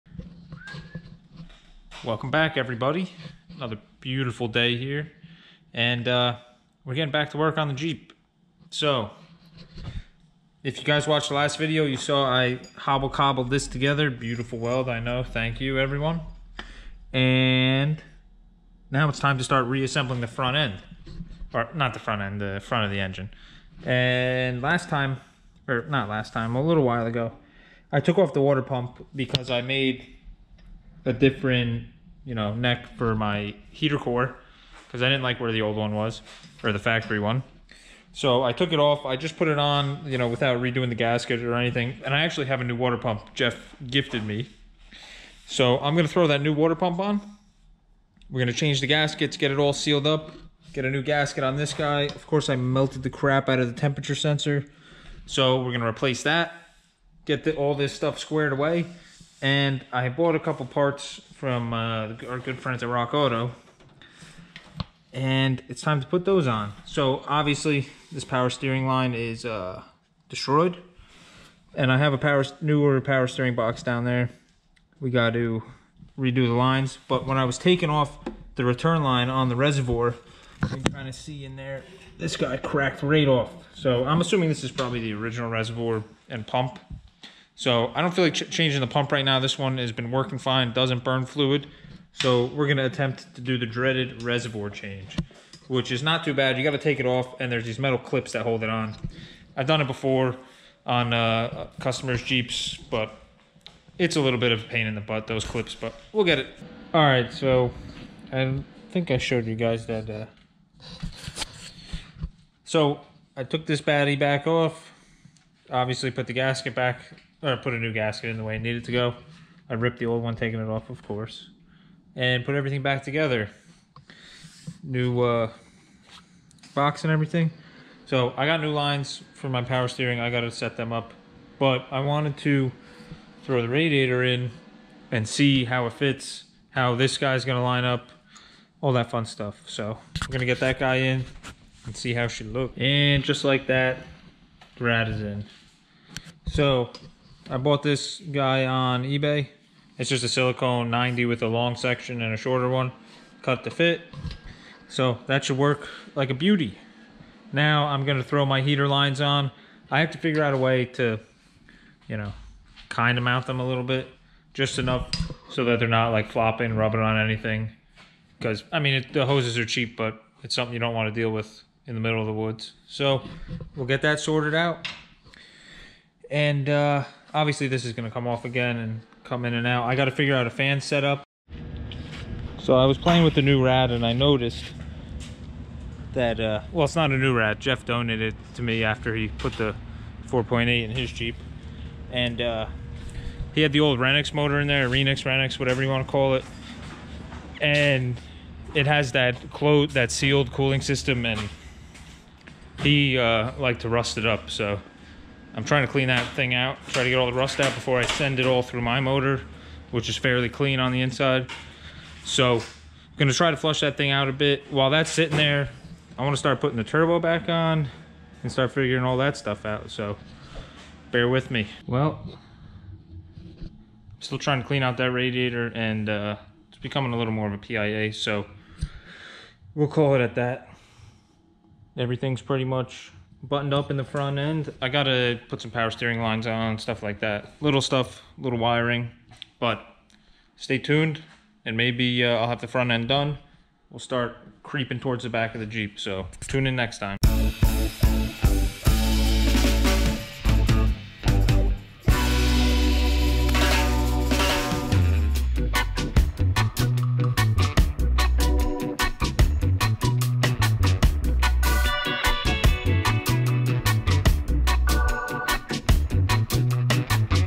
Turbo Jeep Quickie
We are finally getting back to tinkering on the Jeeps! Wrenching season is upon us!

Jeff- @jeff_the_heff
Matt- @flipboard7431
Channel- @gruesomegarage

Suspension:
  -FOA 2.5 coilovers in the front
  -DIY 3 link with parts from Ruffstuff Specialties and TNT Customs
  -Rubicon Express 3.5 leafs in the back
  -Iron Man adjustable shackles
  -HD offraod no lift relocation boxes

Axles:
  -Front dana 60 kingpin, spool, 5:13, 35 spline outers
  -Rear dana 60, 5:13, yukon grizzly

Engine:
  -ARP head studs
  -Lunati cam
  -mls head gasket
  -LS valve springs
  -Megasquirt Ms3x
  -gt35 ebay special